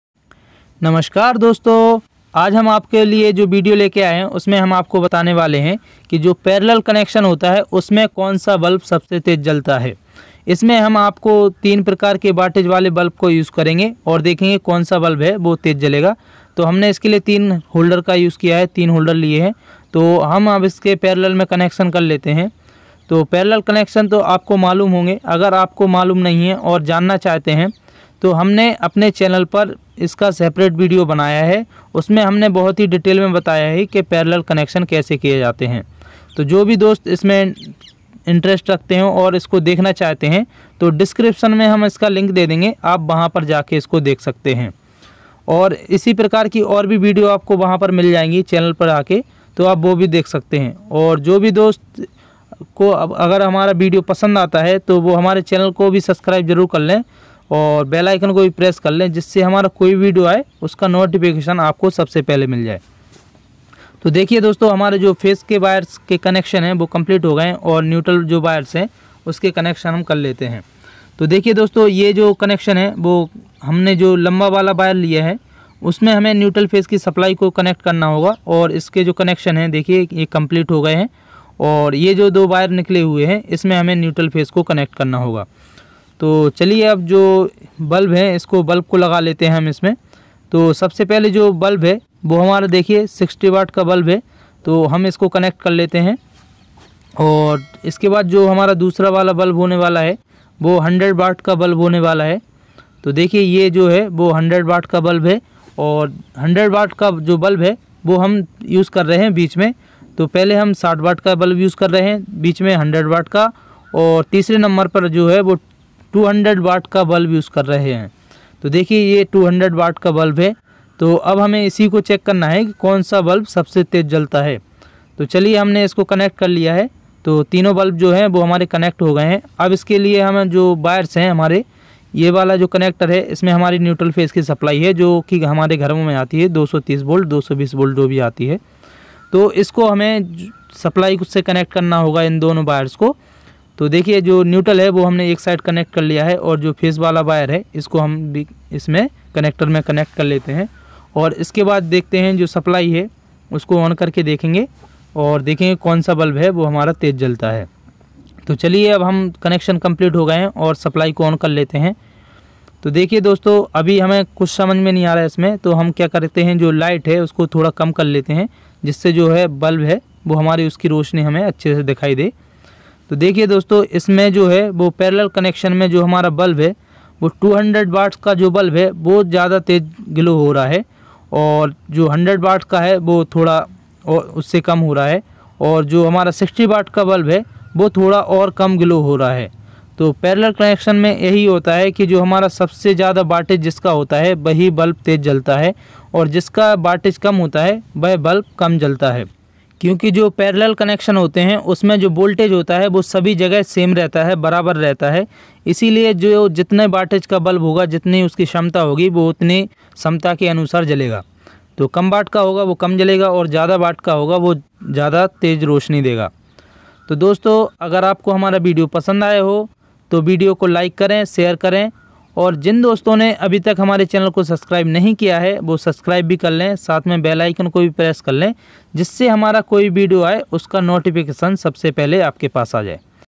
parallel connection | parallel connection me konsa bulb sabse tej jalta hai | iti practical video.
Doston bahut bahut Swagat hai aapka Hamare channel kb electrical maintenance Mein doston Aaj Jo Ham aapke liye video Lekar Aaye Hain usmein Ham aapko batane Wale Hain Ki parallel
connection mein kaun sa bulb Sabse Tej Jalta Hai aur kaun sa bulb sabse kam Jalta Hai Ham ismein alag alag baten wali bulb ka use Karenge aur usmein ham aapko bilkul practical Karke dikhayenge bilkul Aasan tarike se.